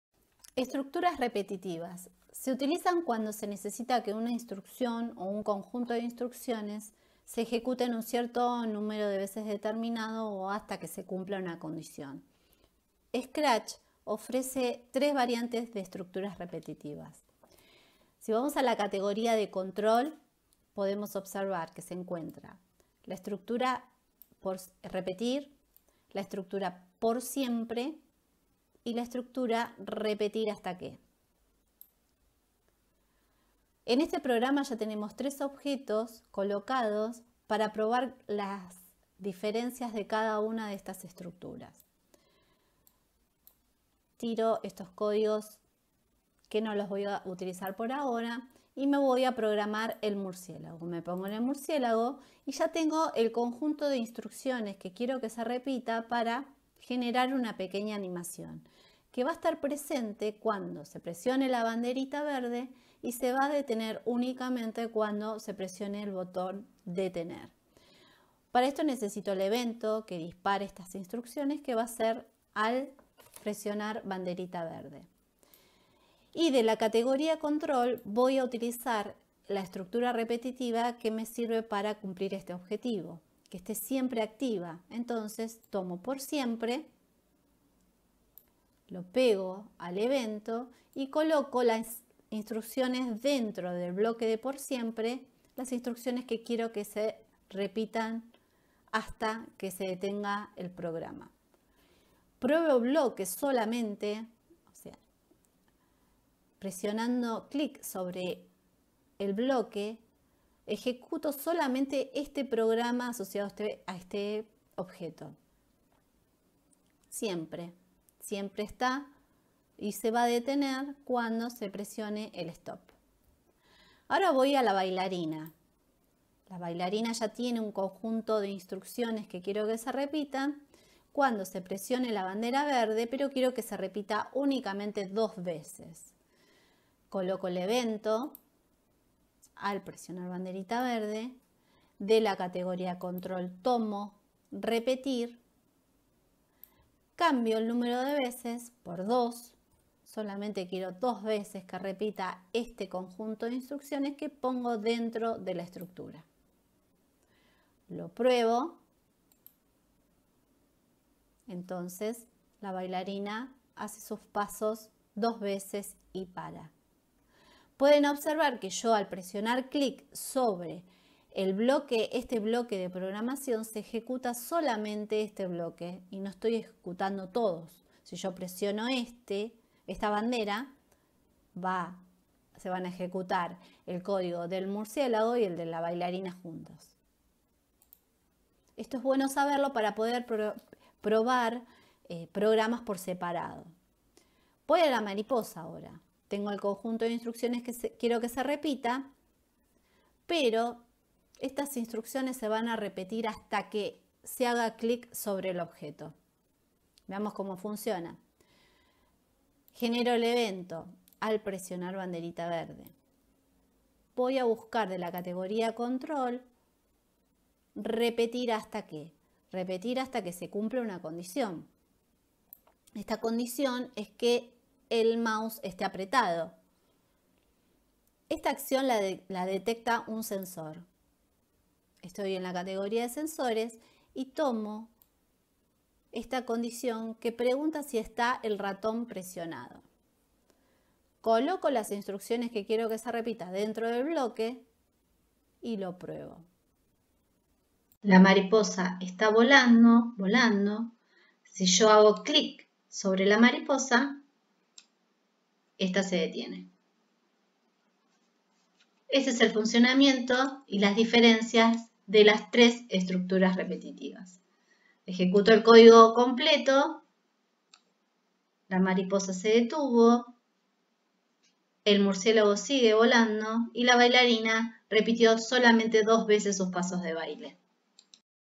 6. Estructuras repetitivas: Tutorial Scratch 3.0
Material diseñado para acompañar los desafíos de programación de "AlgoRitmos".